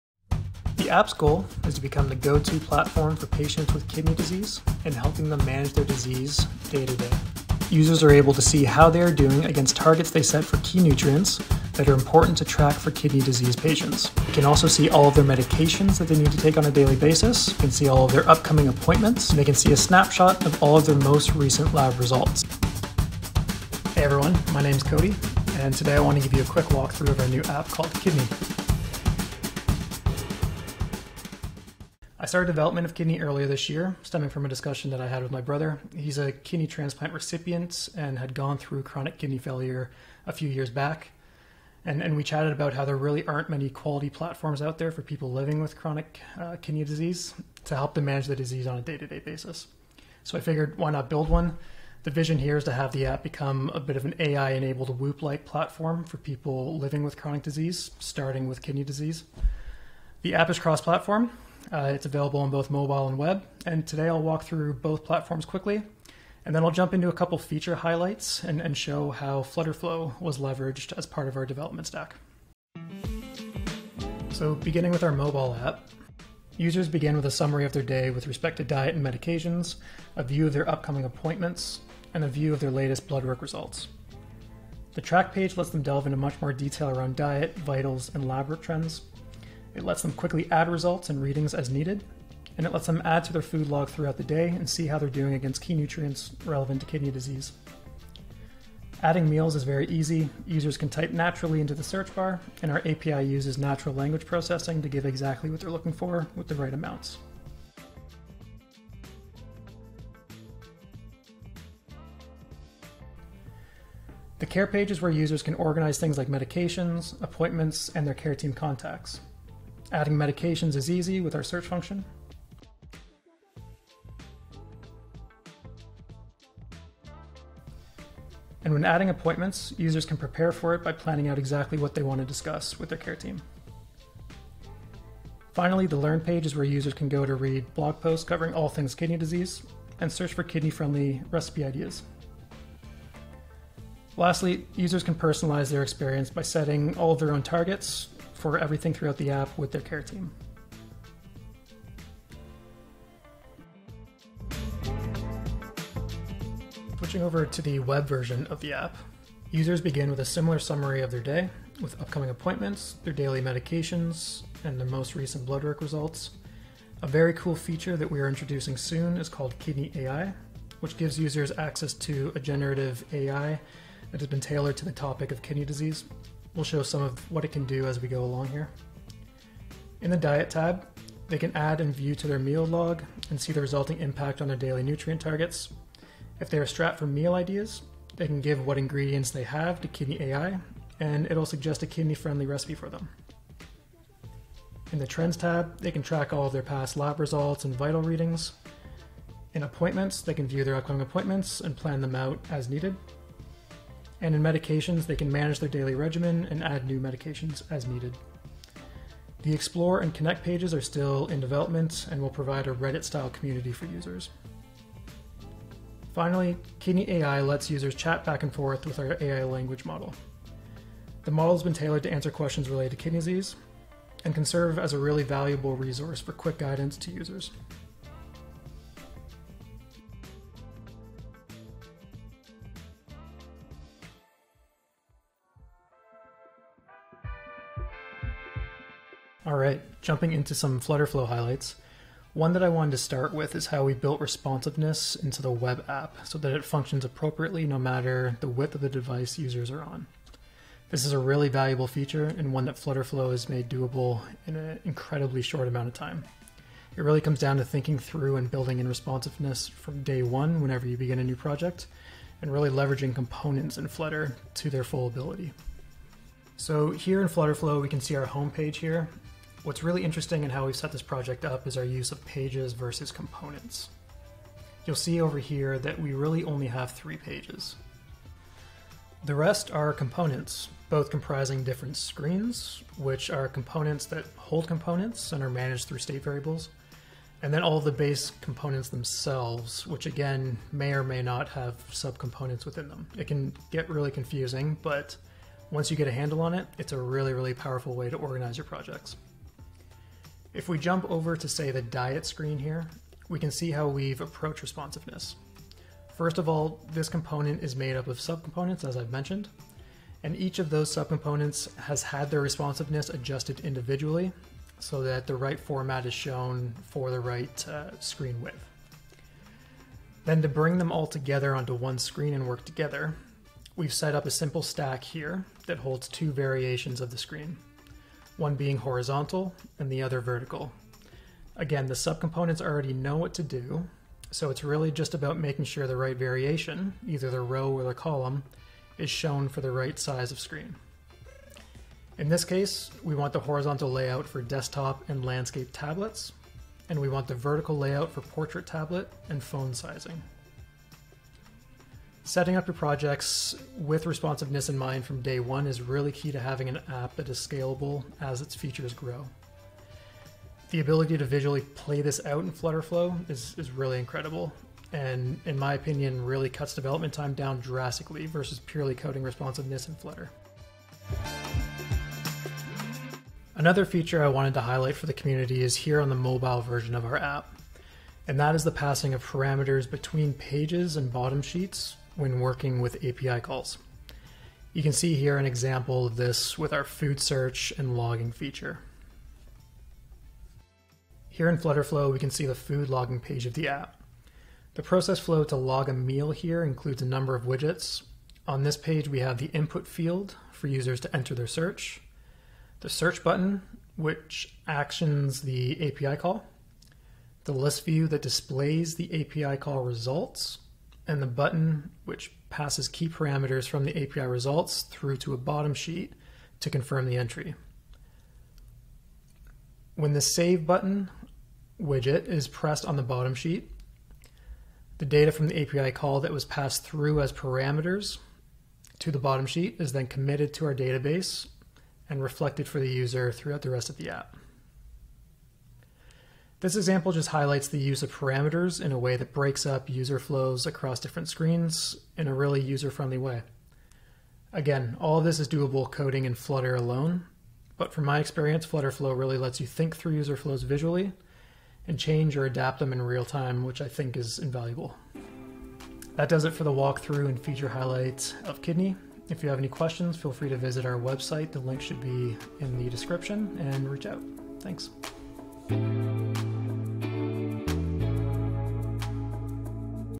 FlutterFlow Founder Highlight - Kidnee, Your Kidney Disease Companion
Welcome to first video in the FlutterFlow Founder Highlight series showcasing an innovative app built in FlutterFlow called "Kidnee" - a kidney disease companion.

Kidnee is an app for people living with kidney disease. This user-friendly app offers a range of features and resources to help manage the day-to-day challenges of kidney disease.

In this video, we take a closer look at Kidnee and explore its key features.  From tracking blood pressure and medication schedules to monitoring nutrient intake and scheduling appointments, Kidnee offers a comprehensive range of tools to help users manage their kidney health.

Cody Hunt, our founder this month, breaks down 2 key FlutterFlow features in his project that can inspire you for your own builds!

If you're interested in learning more about Kidnee and how it can help you or someone you care about manage kidney disease, be sure to watch the video till the end & visit the Kidnee app today.

Breakdown
0:00 Intro
0:31 Founder Story
1:12 Mobile Features
2:35 Web Features
3:57 Feature Breakdown - Responsive Design
6:11 Feature Breakdown - Passing Parameters with API Calls
7:35 Conclusion

If you want to be featured in our next FlutterFlow Founder Highlight, submit your story